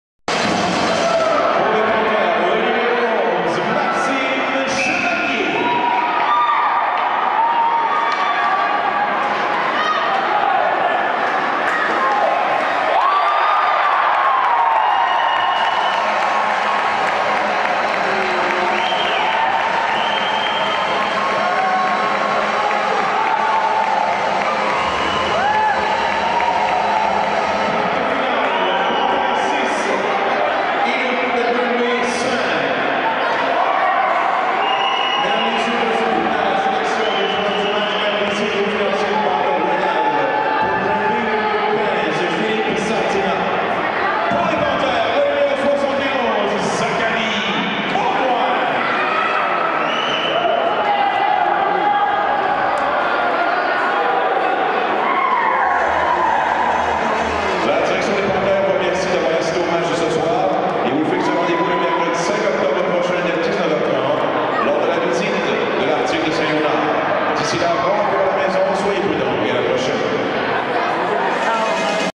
But gagnant de Maxime Chevalier contre Inouk de Granby
Voyez le but gagnant compté en tir de barrage par le défenseur Maxime Chevalier dans une victoire de 6-5 sur les Inouk de Granby.